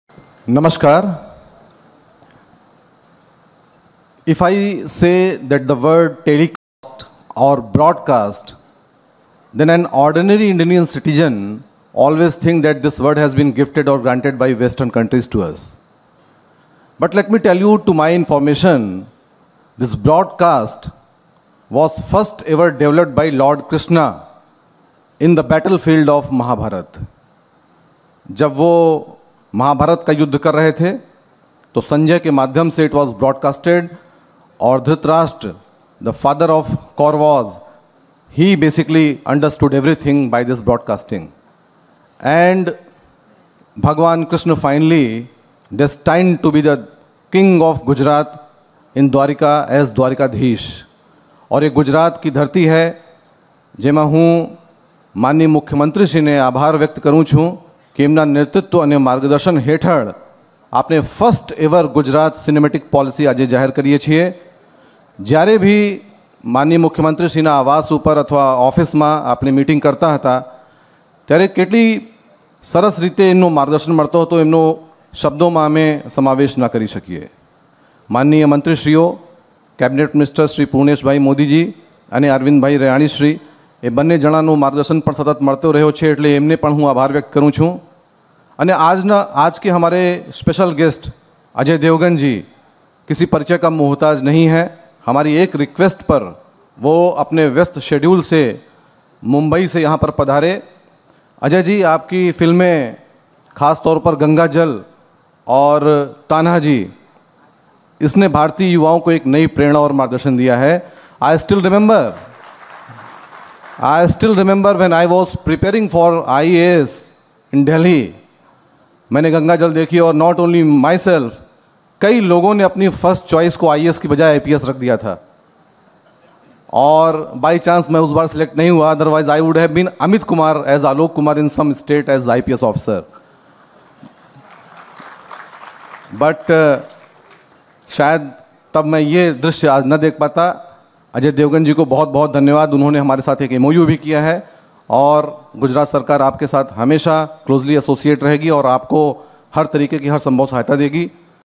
Cinematic Tourism Policy | Gujarat Tourism | Ajay Devgn | Alok Kumar Pandey IAS
At the launching of Gujarat's first-ever Cinematic Tourism Policy by Honorable CM Gujarat and in presence of Shri Ajay Devgn.